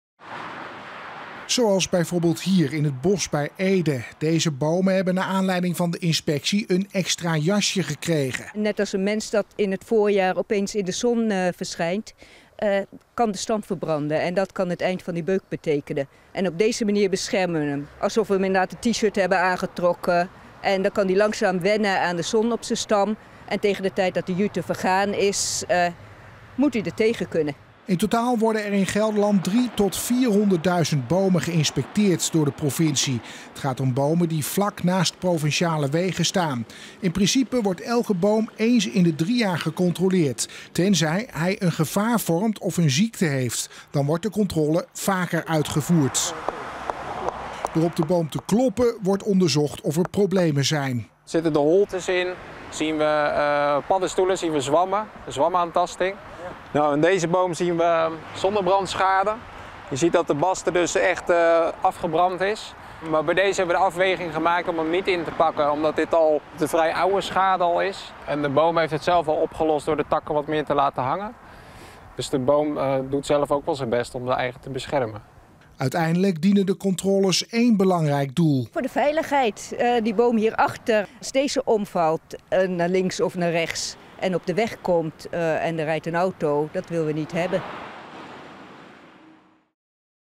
Variatie en zonnebrand moet bomen helpen bij veranderend klimaat
ARNHEM - Het veranderende klimaat heeft ook zijn effect op de bomen in Gelderland. Meerdere droge zomers zijn funest voor sommige soorten. De provincie houdt daarom extra inspecties bij bomen die daar veel last van hebben. En kilometers lange lanen met dezelfde soort bomen maken, bij nieuwe aanplant, plaats voor lanen met verschillende bomen.